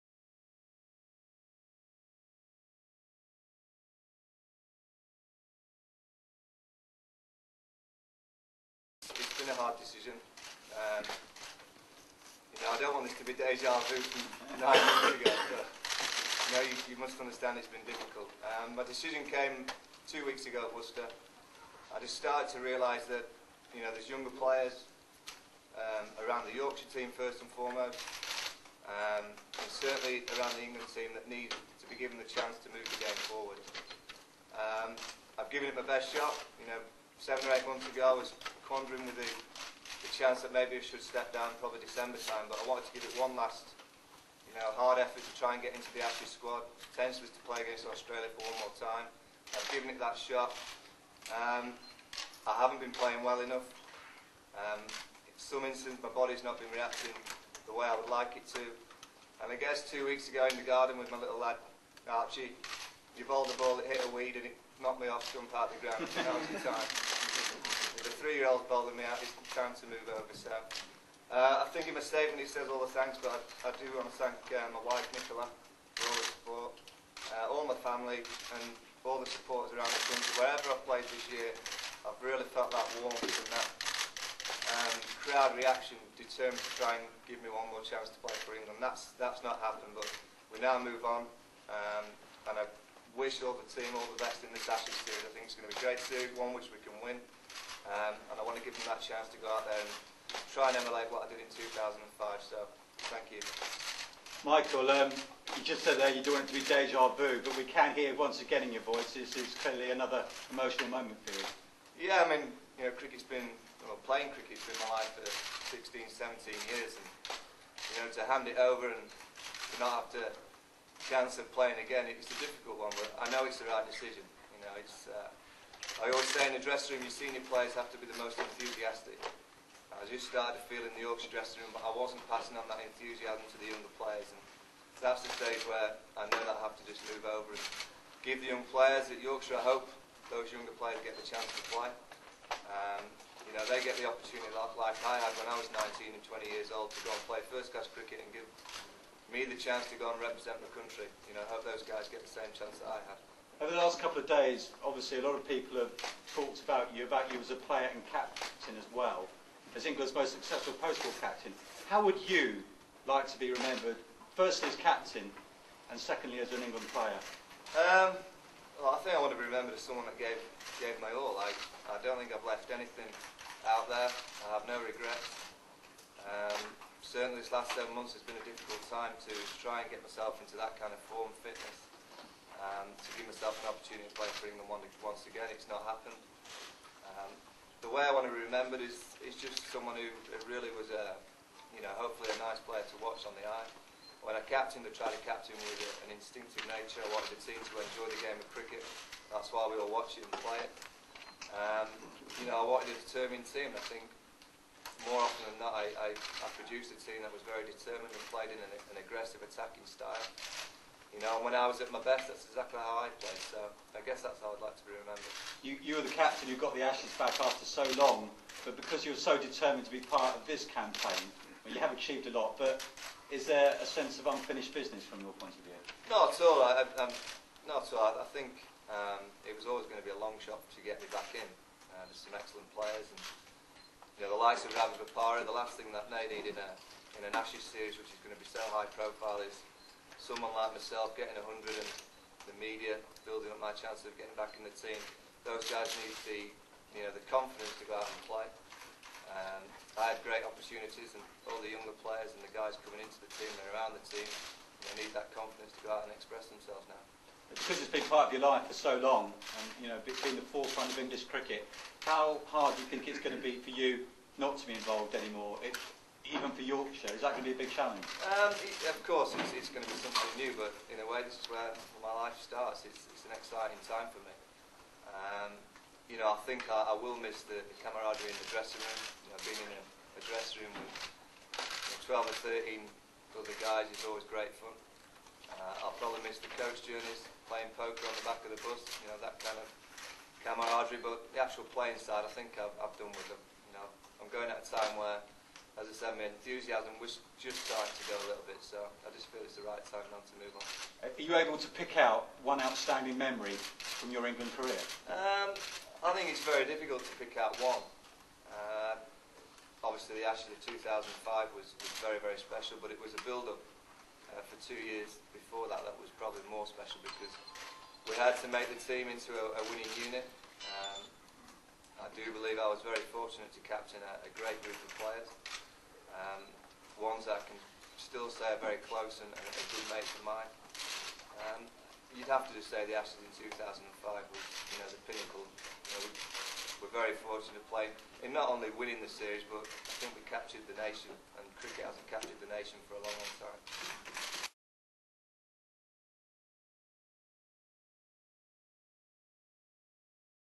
Vaughan retires
Former England captain Michael Vaughan retires from professional cricket with immediate effect